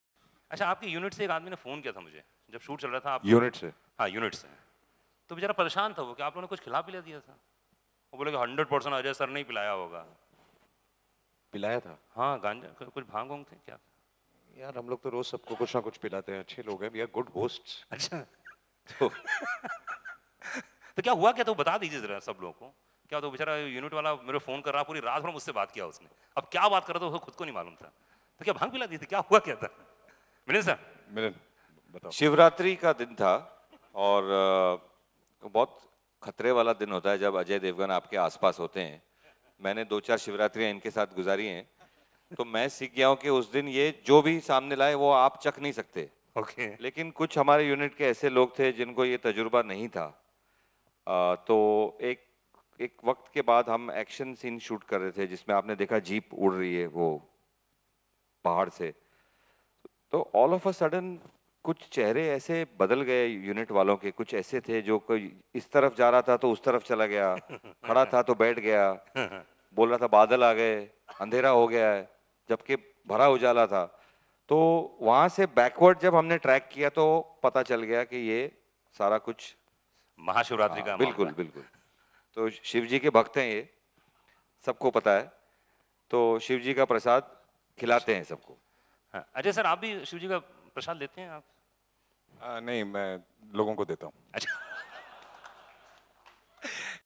Ajay Devgun Ki Set Par Masti Ka Kissa - Sari Unit Ko Pila Di Bhang - Must Watch
Funny Incident Story of Ajay Devgun -  Ajay Devgun Ki Set Par Masti Ka Kissa - Sari Unit Ko Pila Di Bhang - Must Watch